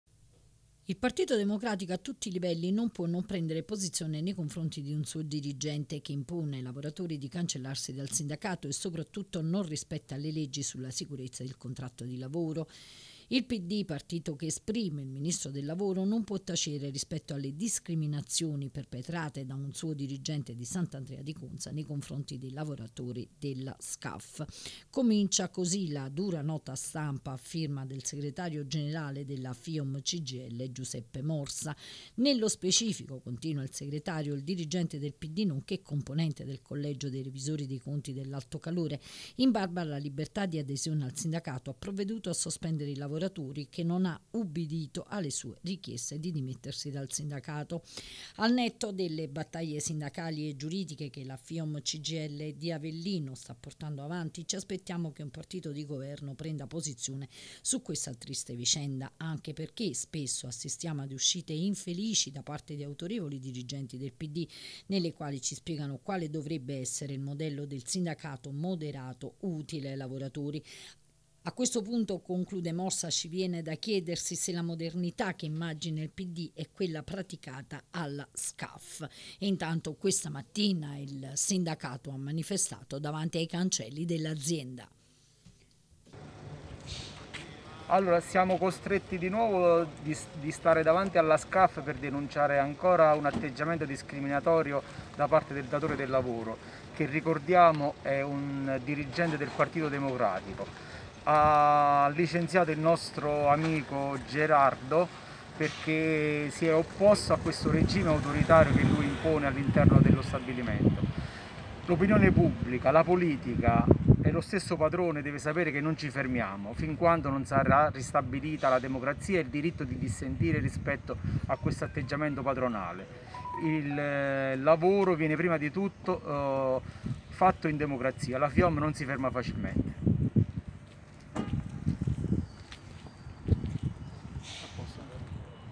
Operaio Scaf licenziato . Il sindacato manifesta e chiama il Pd
Duro attacco del segretario fella Fiom Cgil, Giuseppe Morsa, dopo il licenziamento dell’operaio della Scaf di Morra De Sanctis. Il titolare dell’azienda è un dirigente del Pd, stamane sit in di protesta del sindacato davanti allo stabilimento